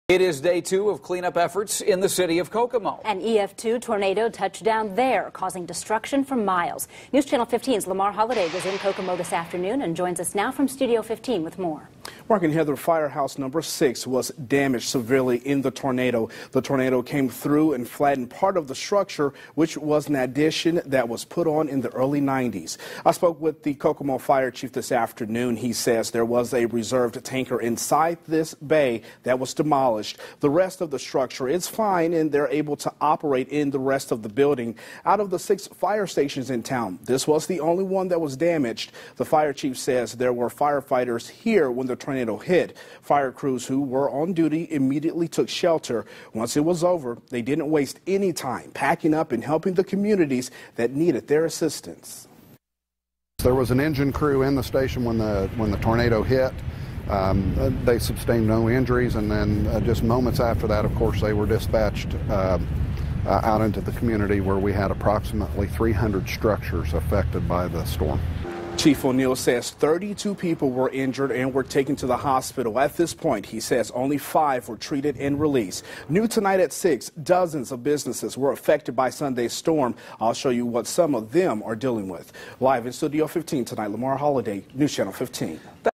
Firehouse in Kokomo hit by Sunday's tornado
Kokomo Firehouse 6 was damaged by Sunday's tornado. After the storm passed, fire crews went out and helped surrounding communities.